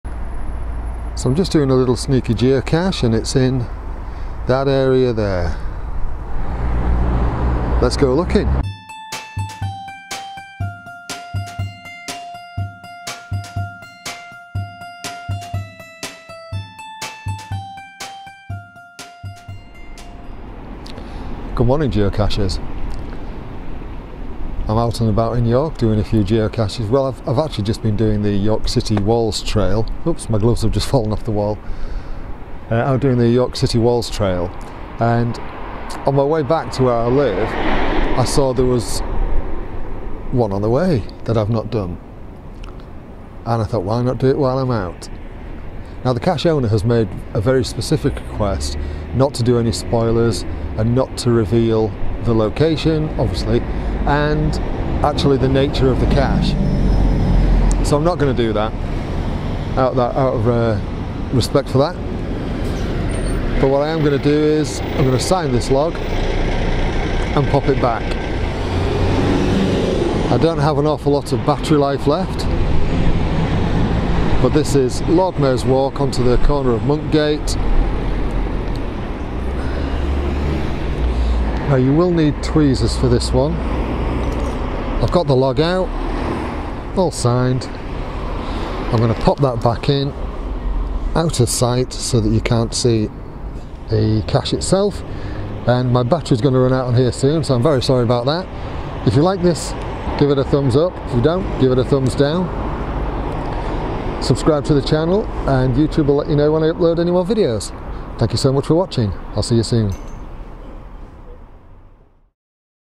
𝙂𝙚𝙤𝙘𝙖𝙘𝙝𝙞𝙣𝙜 𝙐𝙆 - 𝙏𝙝𝙚 𝙇𝙤𝙫𝙚 𝙎𝙚𝙖𝙩
Filmed on the last day of January I went looking for this geocache on my way home from completing the series around the York City Walls.